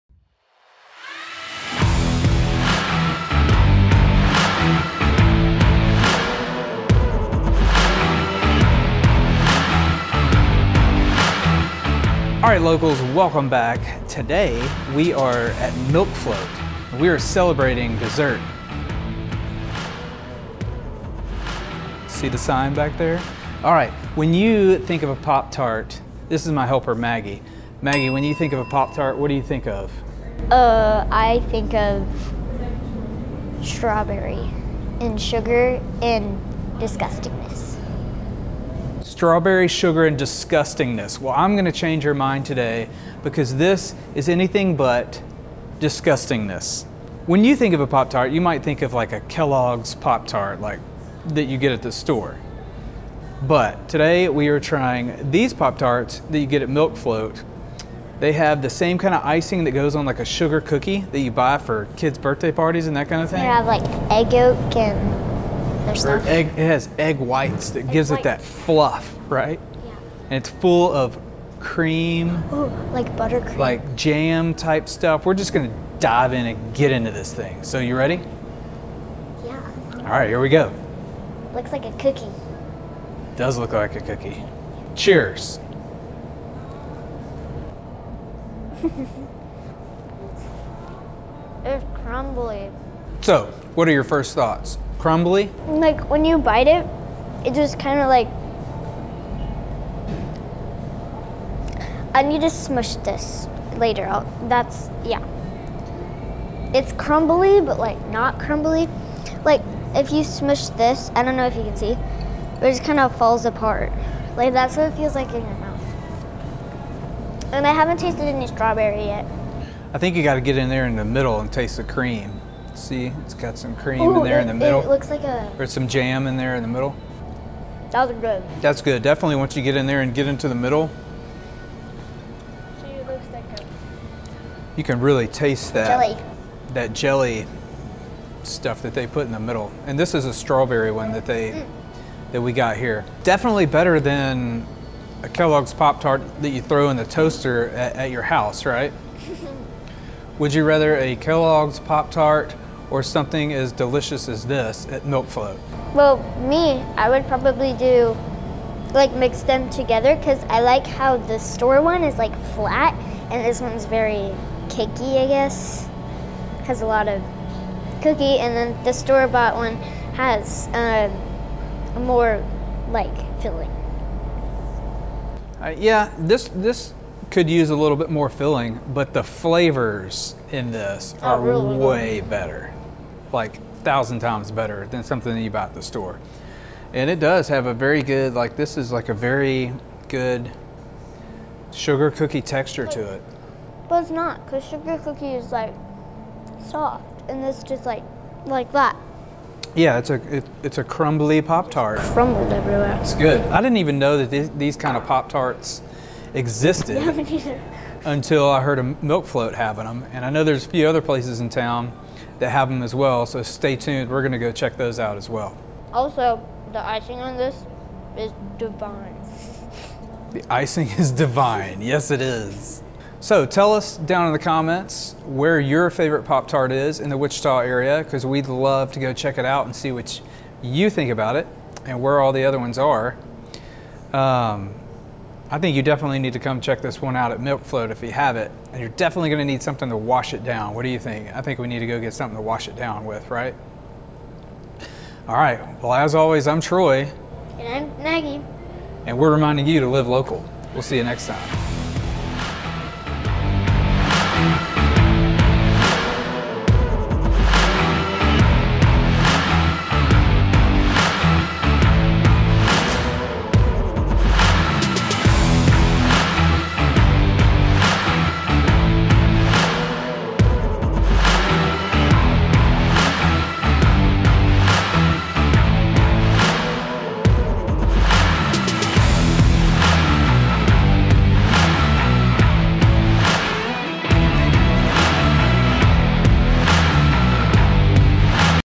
Homemade Strawberry Pop Tart Food Review at Milkfloat | Live Local Wichita
We sat down at Milkfloat to try their homemade strawberry pop tarts.

At Live Local, we love eating local food! Our conversation about food is pretty hilarious! We have a great time, so join us on our adventures!